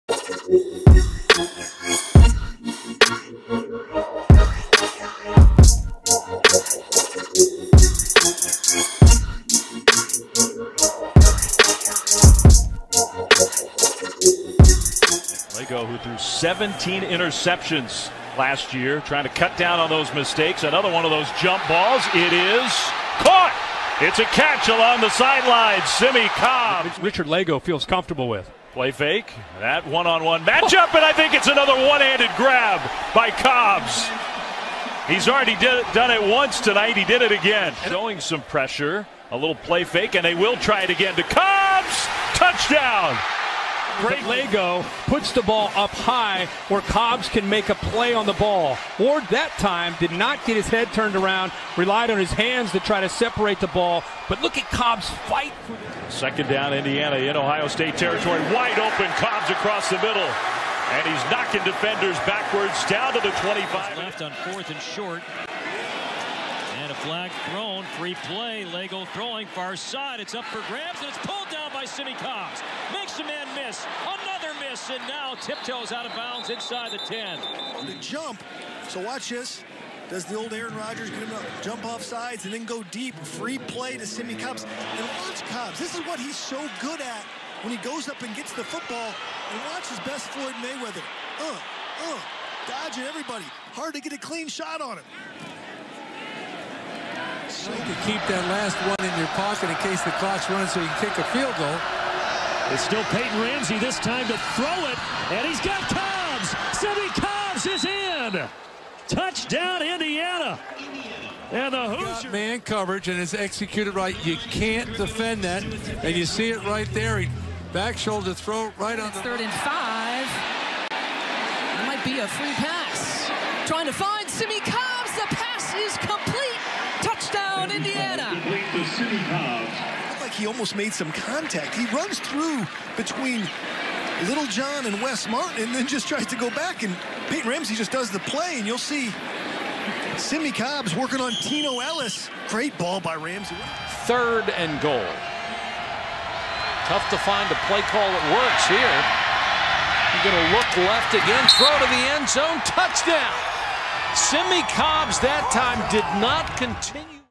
Simmie Cobbs Indiana 2017 Season Highlights ᴴᴰ || "Welcome to Washington"
Highlights from Indiana WR Simmie Cobbs 2017 season.